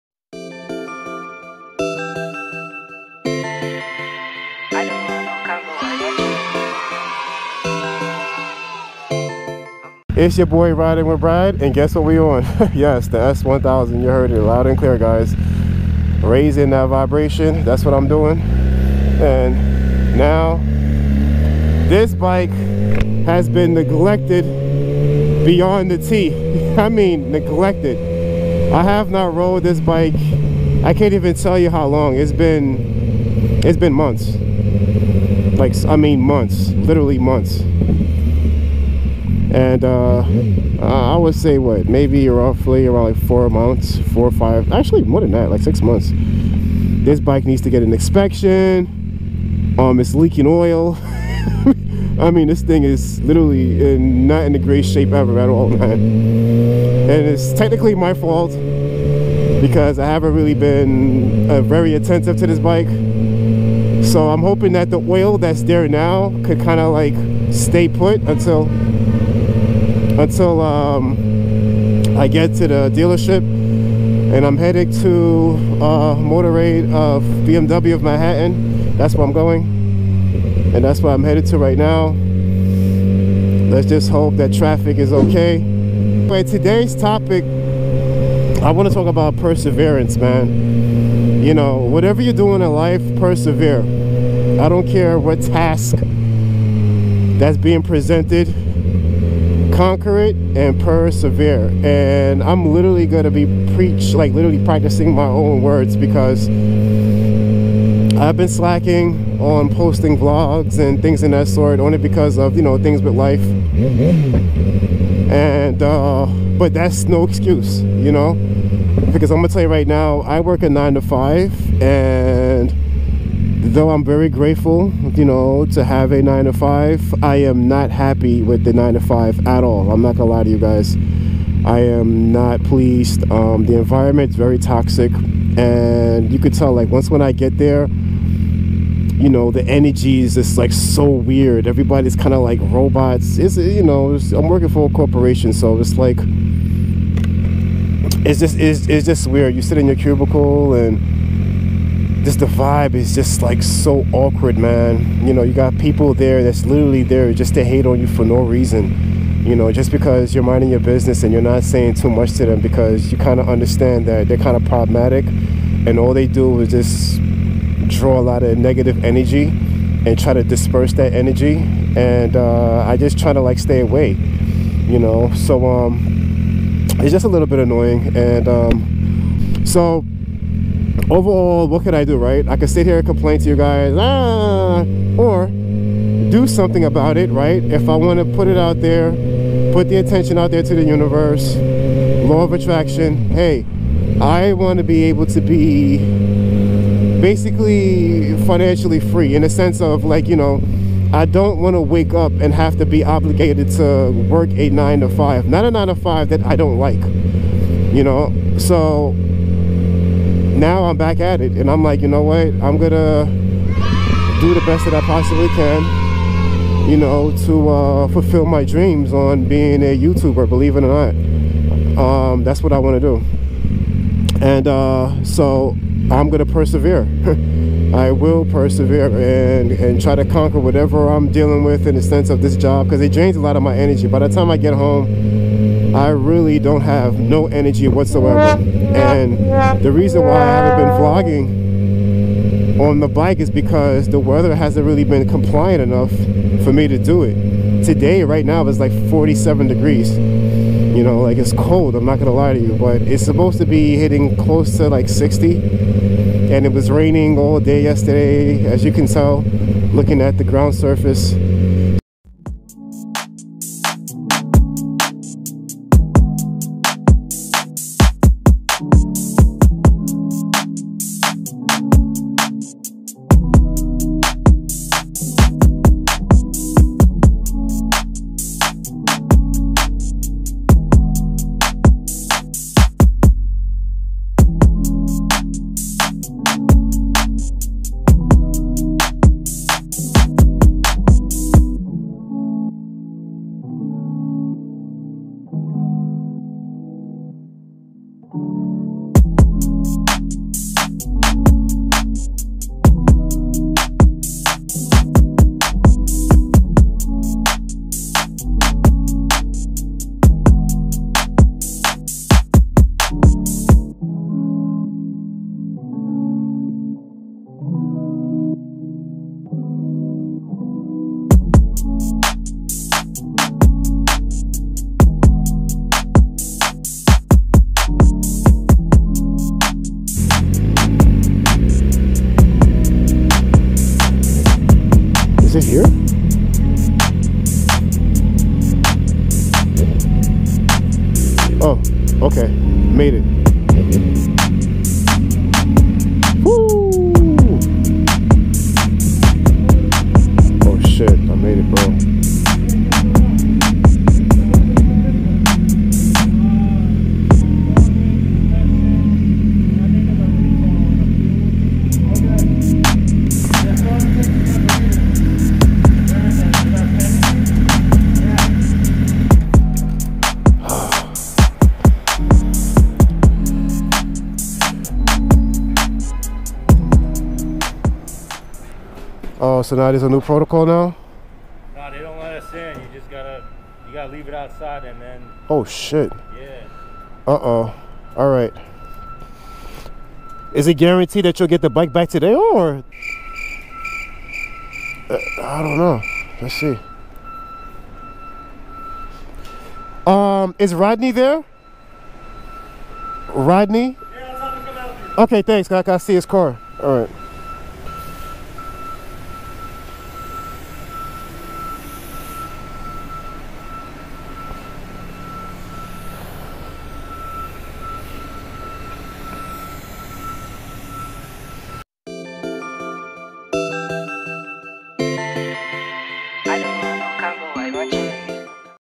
Perseverance
Today finally a motovlog sorry I've been experiencing alot lately life and little stress at the job. So today I took off to take care of S1000RR brought the bike in for annual service and I realize how stress free I was not dealing with negative 🙃 drama at the office. That was one of the reasons why I was to drain to vlog but now I will over come that challenge and use it was fuel to pursue my dreams to be a vlogger something I love!